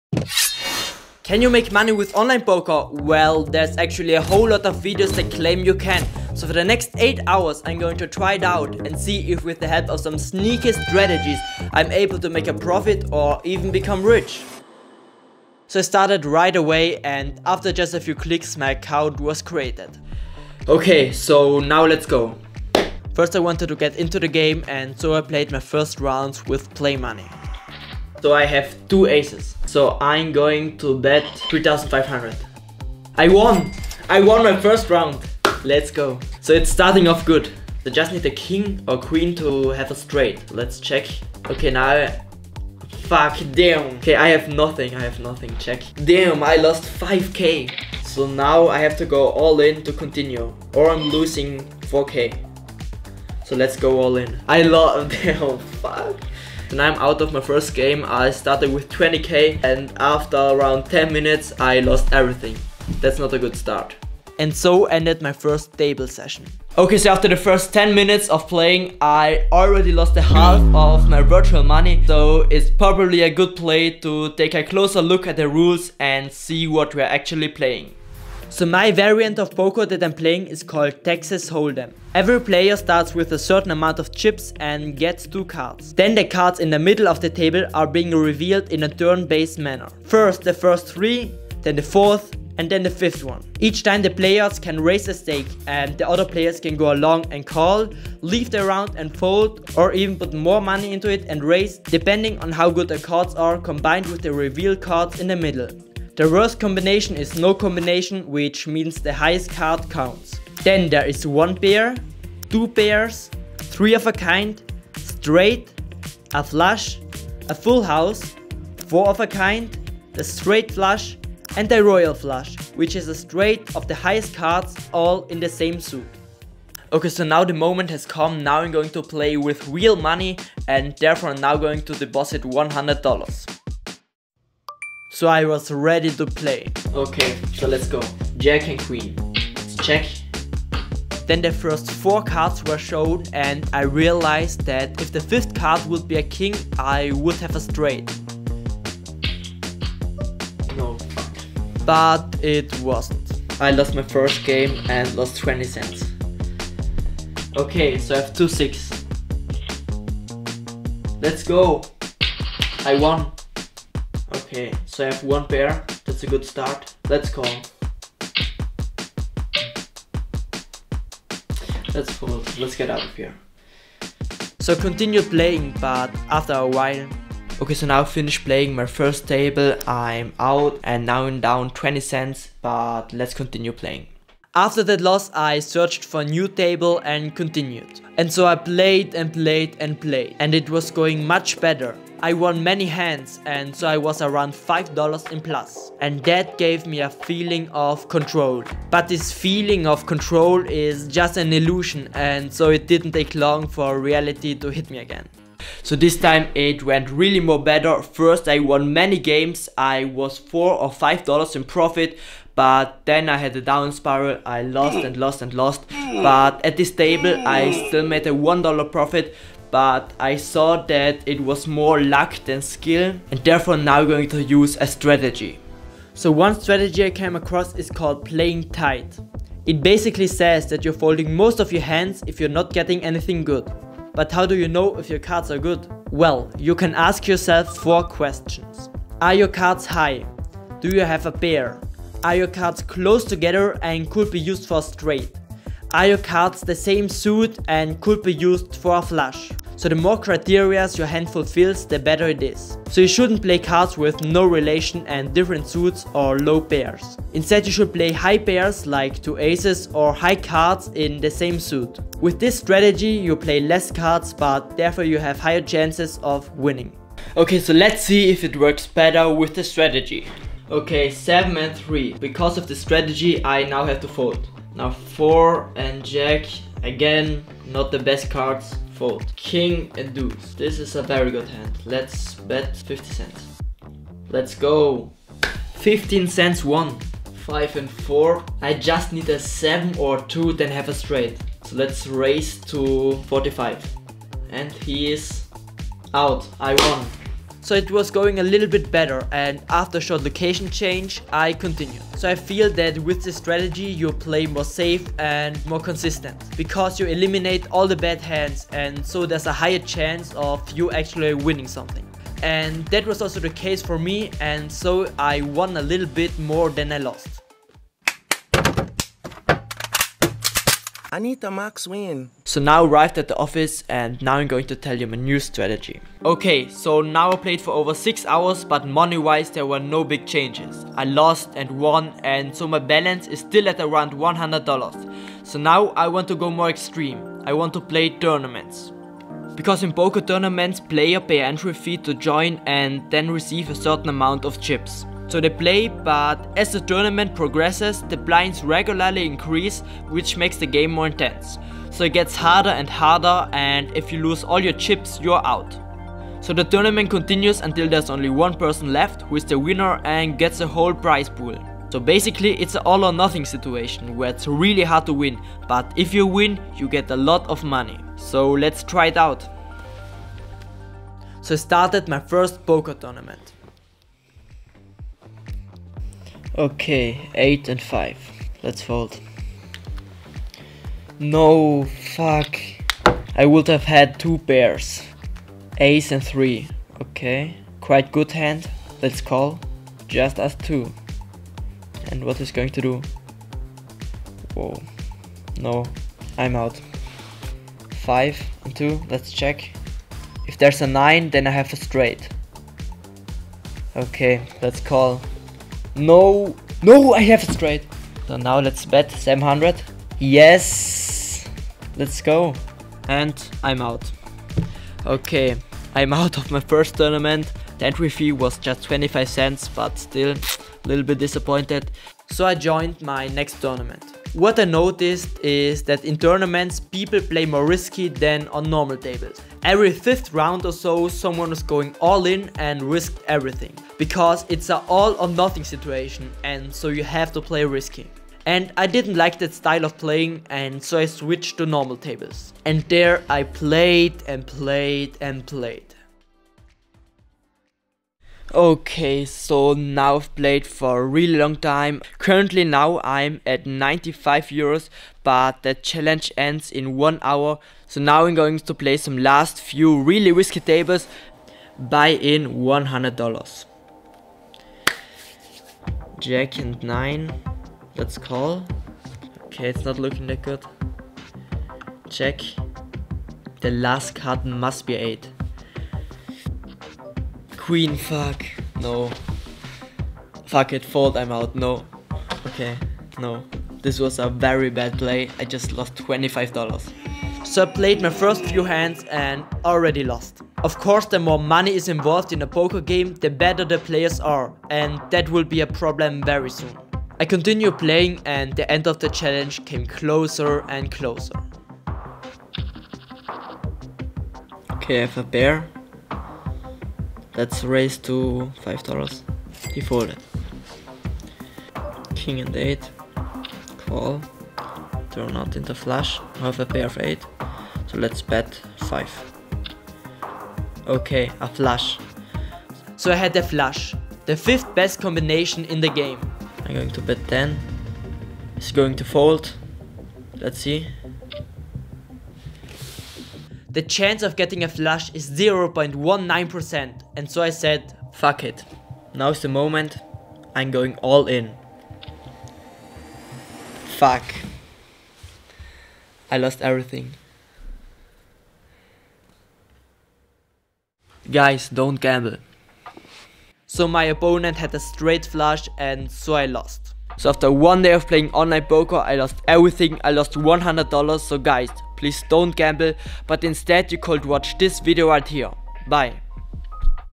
I Tried Online Poker as a Complete Beginner
I tried to make money with online poker for 8 hours.

I'm a complete beginner and just did it for the video. Furthermore, I would never play online poker privately, and would not recommend it to anyone, because if you see the video, you know what can go wrong.

0:00 Intro
0:17 Playing Poker
1:19 Explanation of Poker
2:07 Playing with Money
3:46 Explanation Strategy
4:27 Playing with Strategy
7:41 Finale